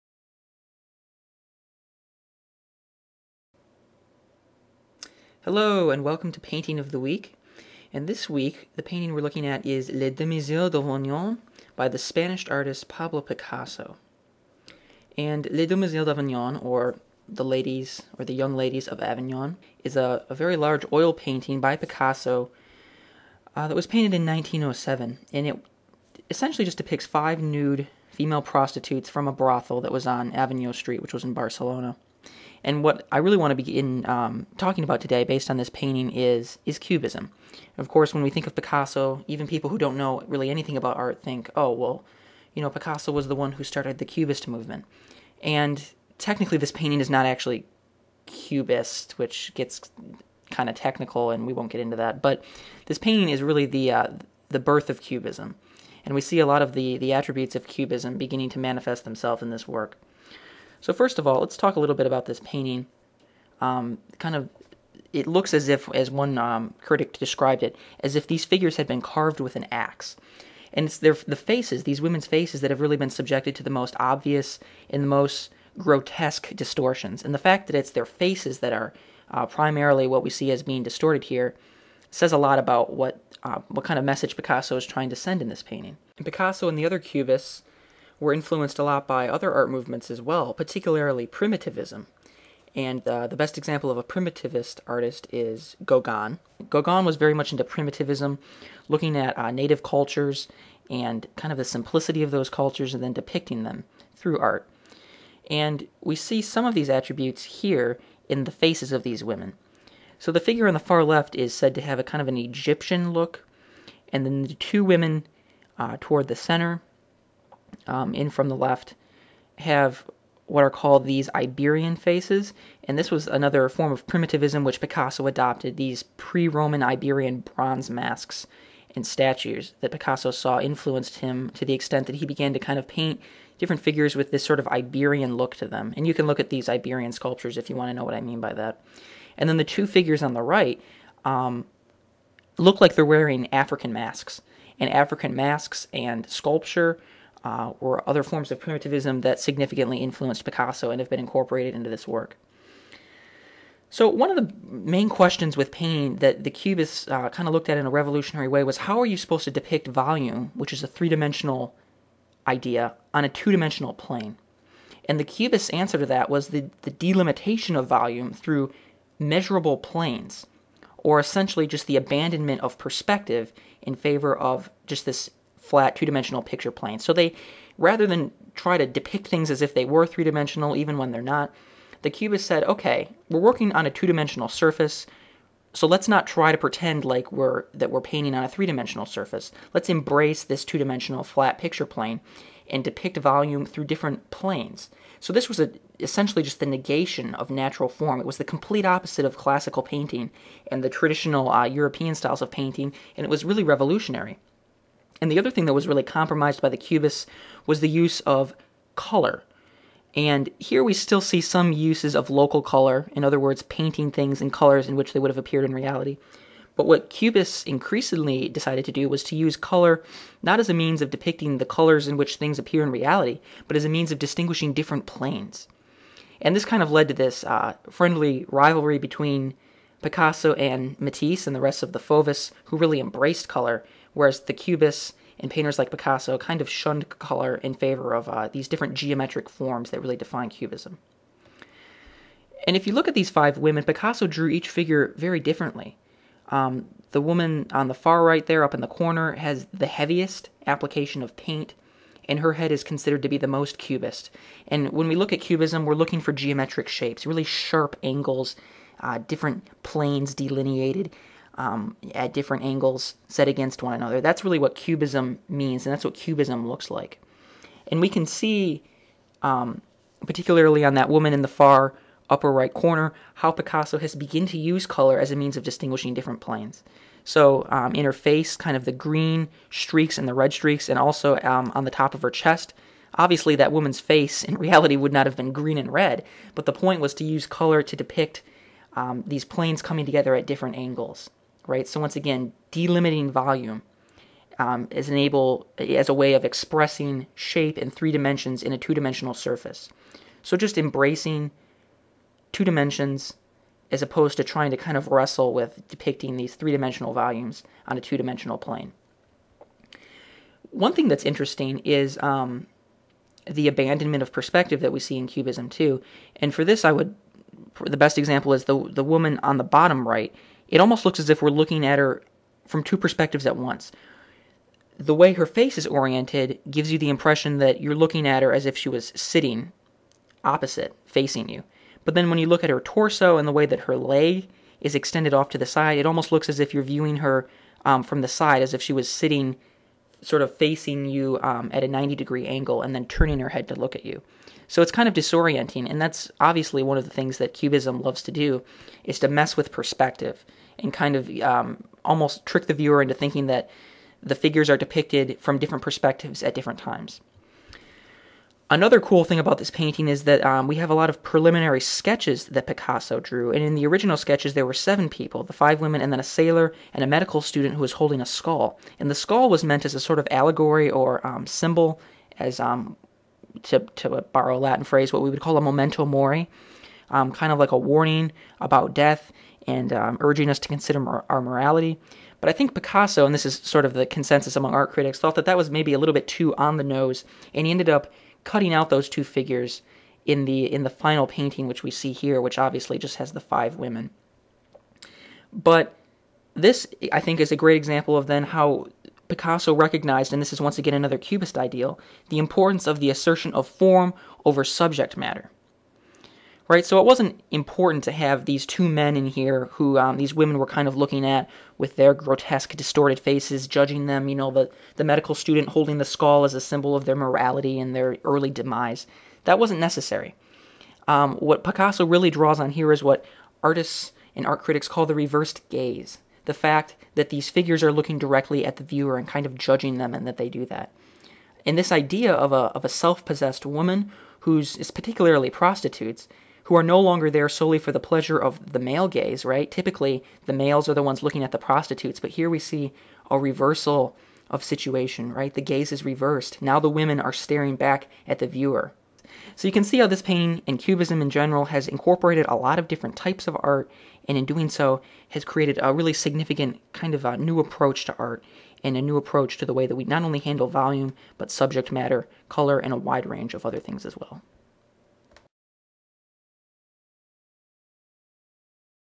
Les Demoiselles d'Avignon (Picasso)
This week learn about the birth of Cubism from a masterpiece by Pablo Picasso